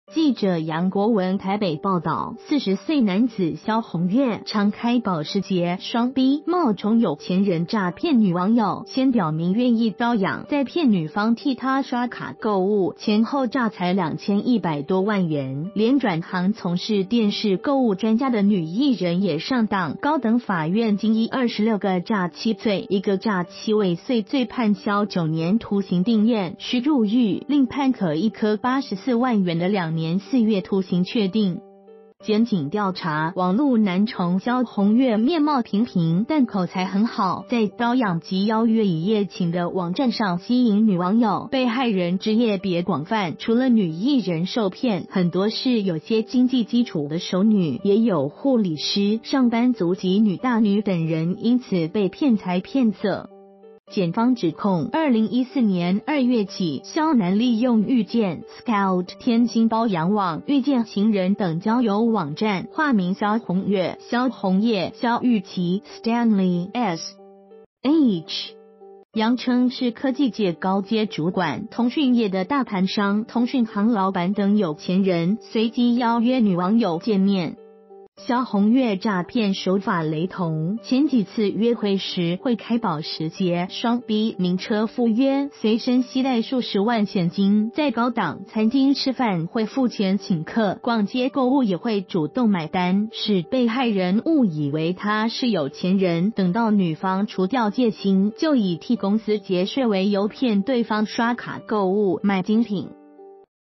禿頭男開雙B裝有錢 騙財騙色女藝人也上當
〔記者楊國文／台北報導〕40歲男子蕭弘岳，常開保時捷、雙B冒充有錢人詐騙女網友，先表明願意包養，再騙女方替他刷卡購物，前後詐財2100多萬元，連轉行從事電視購物專家的女藝人也上當，高等法院今依26個詐欺罪、1個詐欺未遂罪判蕭9年徒刑定讞，須入獄，另判可易科84萬元的2年4月徒刑確定。

發表評論評論如下。